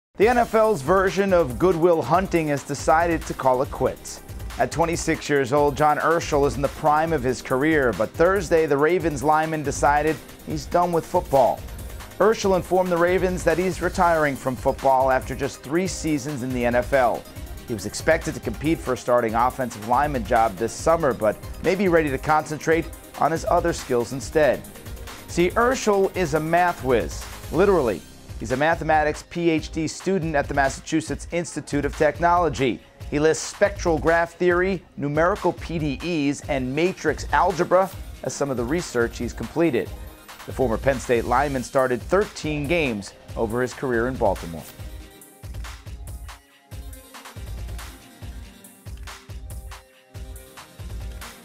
Baltimore Ravens' John Urschel Announces His Retirement At Age 26 | SI Wire | Sports Illustrated
Baltimore Ravens offensive lineman John Urschel has announced his retirement from the NFL at the age of 26.